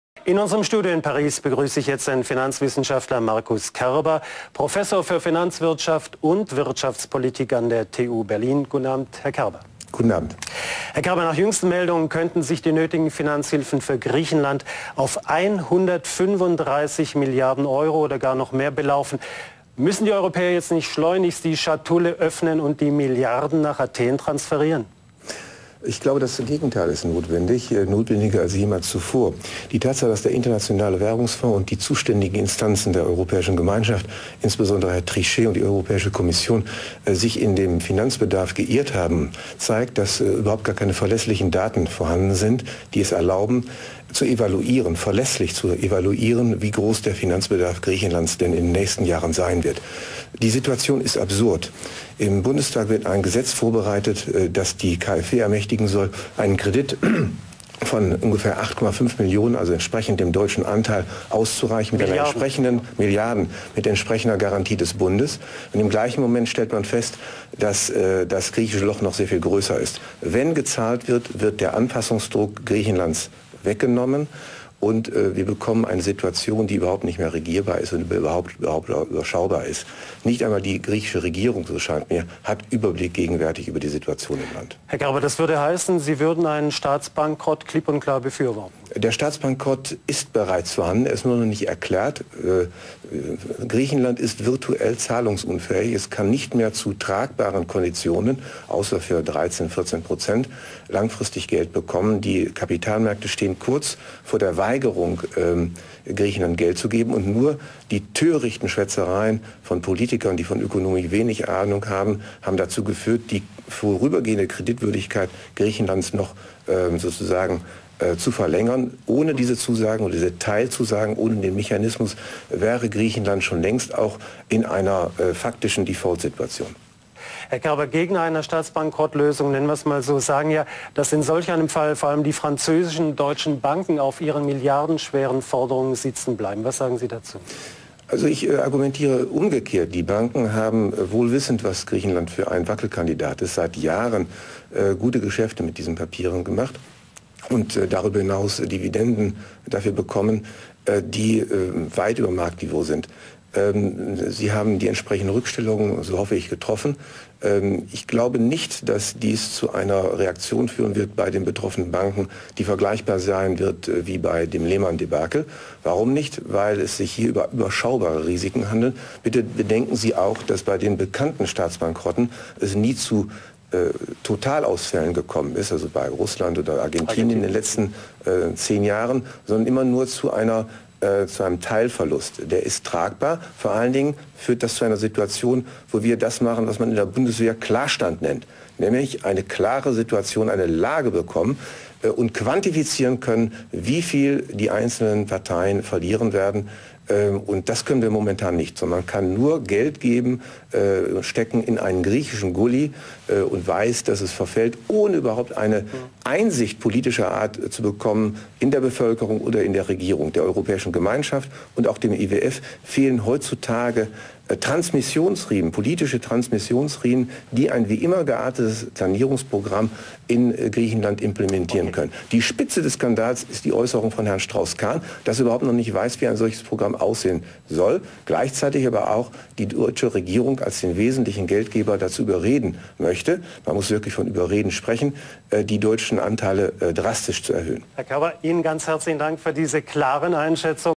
Prof. Markus c. Kerber zur Griechenland"hilfe"
Die illegale "Finanzhilfe" nützt ausser den Bänkern keinem, auch nicht den Griechen! wieder einmal geht es um die Umverteilung von Besitz